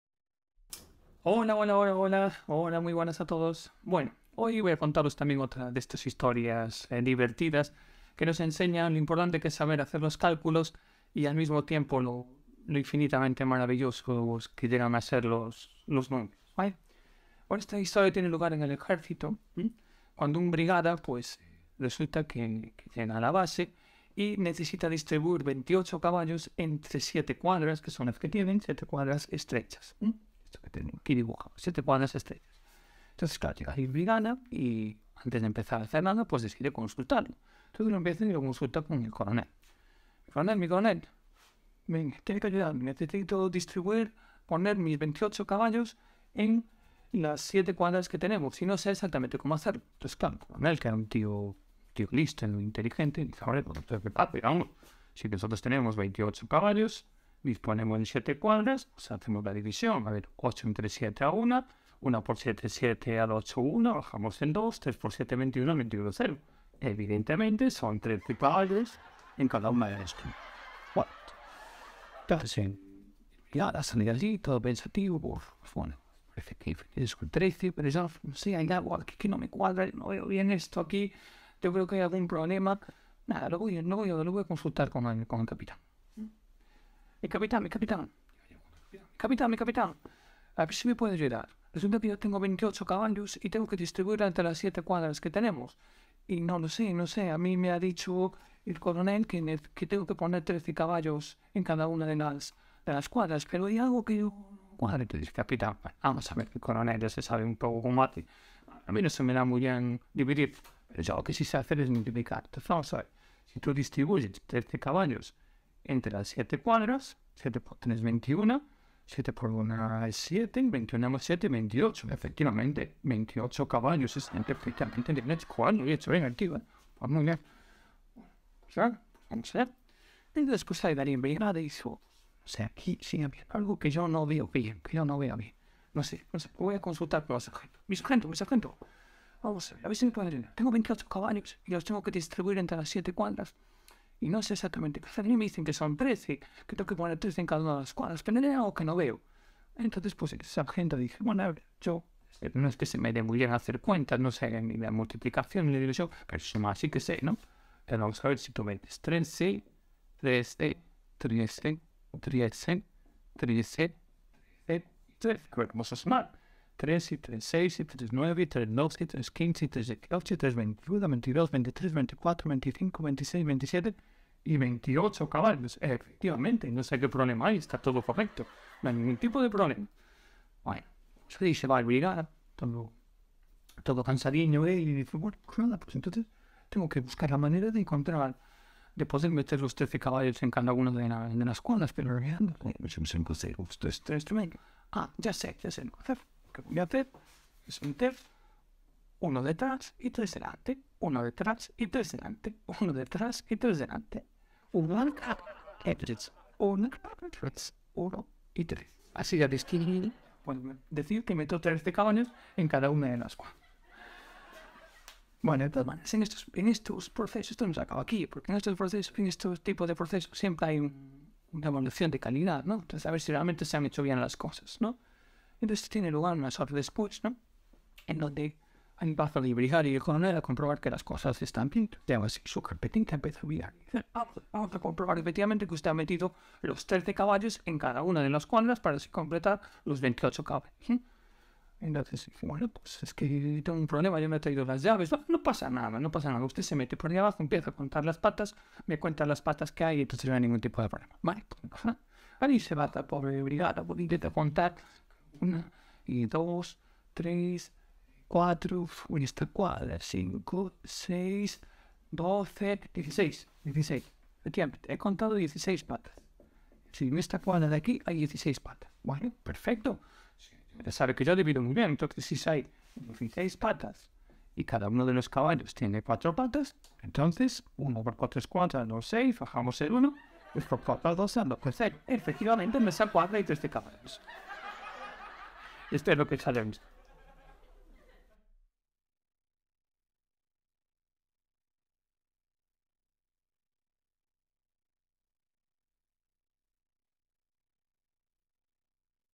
CHISTE MATEMÁTICO SORPRENDENTE.Cómo repartir 28 caballos en 7 cuadras🤣🤣
CON LA AYUDA DE ESTE CANAL MÁS DEL 90% DE LIS ALUMNOS APRUEBAN MATEMÁTICAS CON BUENA NOTA.
Humor y matemáticas, la importancia de hacer bien las cuentas, chiste de matemáticas.Las matemáticas son divertidas y útiles y fáciles.

Ojalá que os guste

También te puedes pasar por mi web